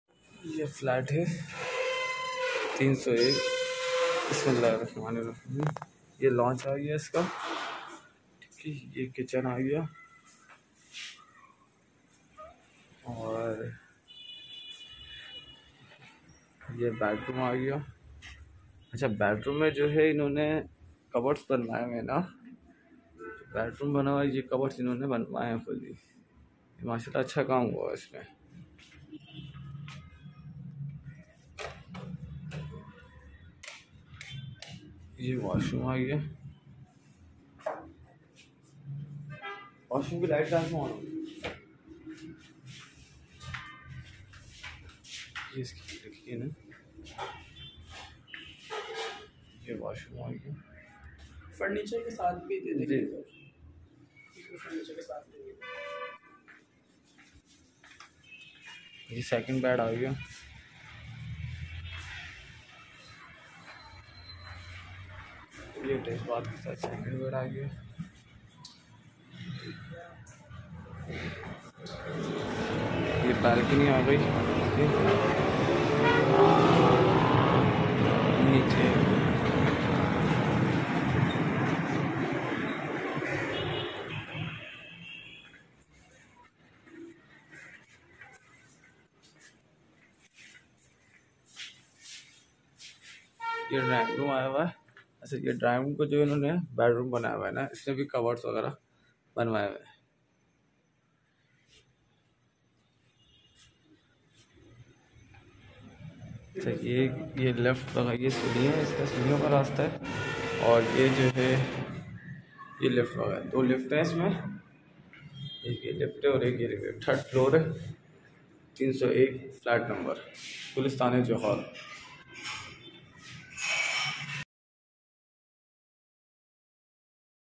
AL DINAR RESIDENCY GULASTAN-E-JOHAR KARACHI 2 BED DD FLAT AVAILABLE FOR SELL
FLAT AVAILABLE IN AL DINAR RESIDENCY GULASTAN-E-JOHAR KARACHI

1050 SQ FT
2 BED DD
3 FLOOR
LIFT STAND BY GENERATOR
NEW CONSTRUCTION
NO NEED OF WORK REQUIRED
JUST BUY AND LIVE
ATTACHED BATH ROOMS

FOR VISIT AND DETAILS CONTACT

SKY PROPERTIES